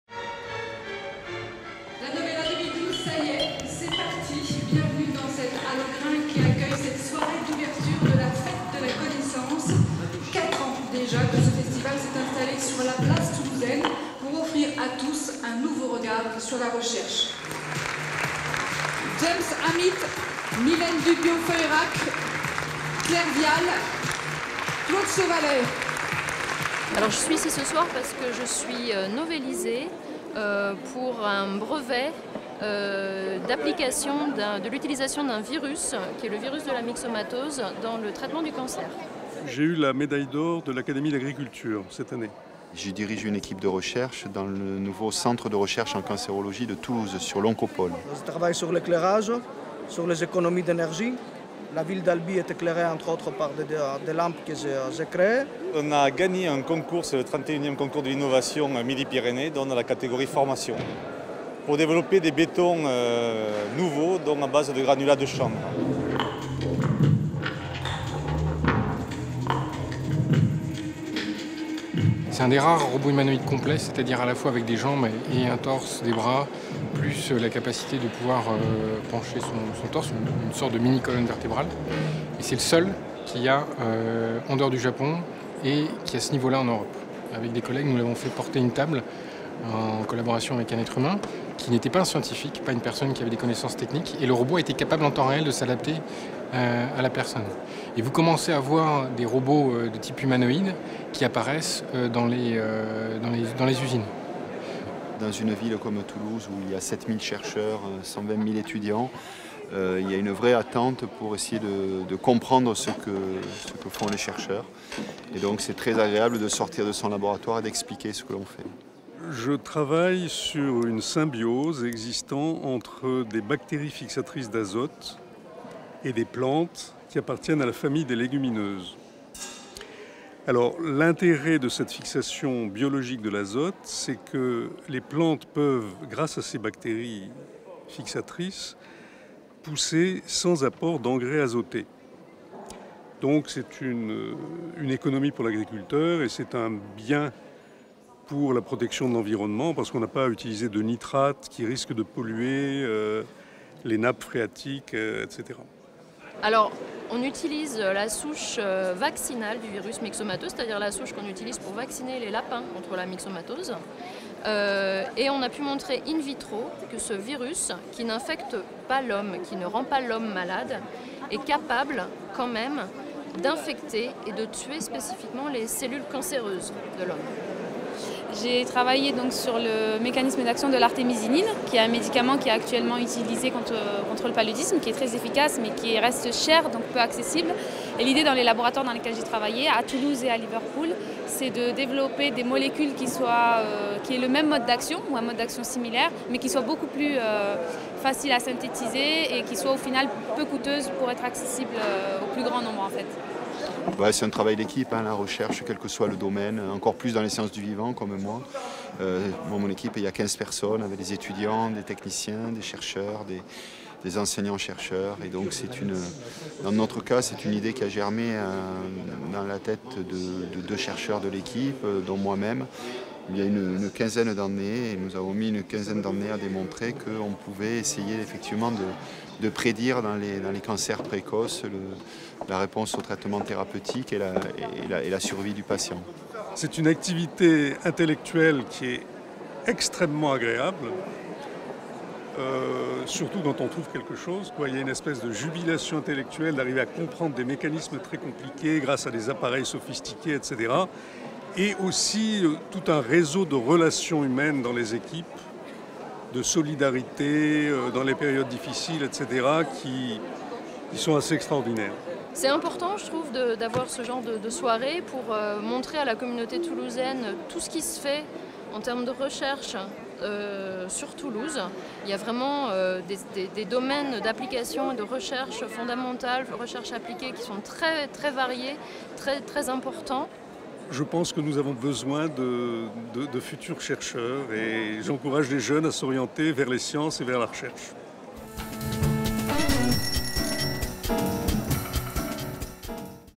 La Soirée des Novélisés, Toulouse le 5 octobre 2012
La soirée des Novélisés qui ouvre La Novela, rassemble et fait connaître à un public populaire les chercheurs des laboratoires toulousains qui ont reçu des récompenses académiques. Toulouse est la seule ville de France à honorer ainsi ses chercheurs reconnus.